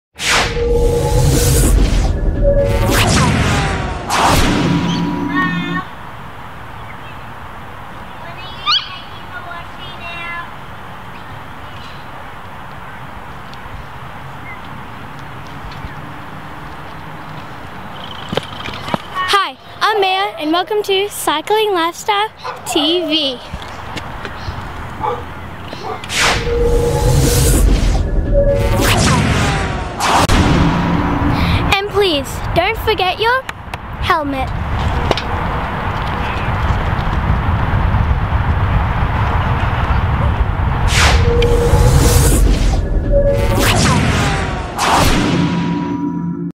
MAYA safety message for Cycling Lifestyle TV
Maya delivering a simple safety message to kids on bikes